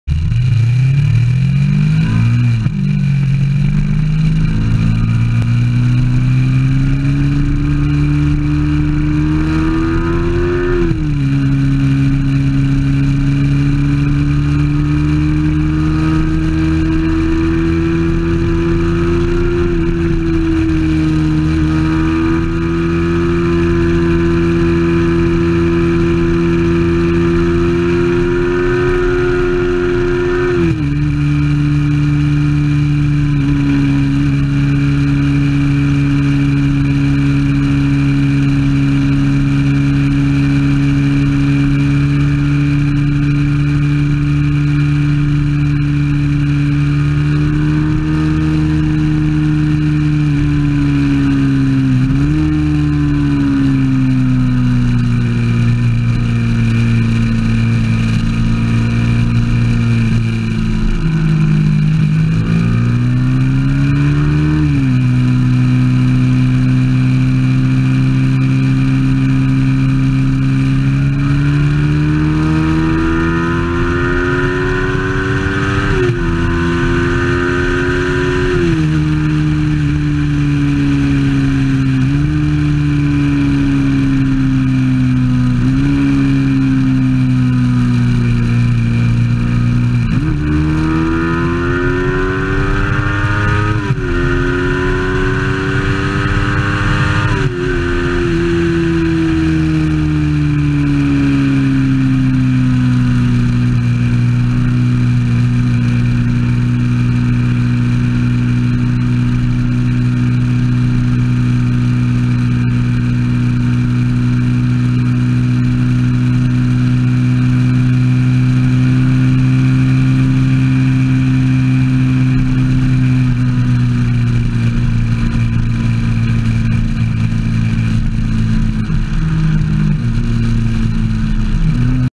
Follow Cam Of Some Wheelies In Mexico
Just some good wheelies in mexico!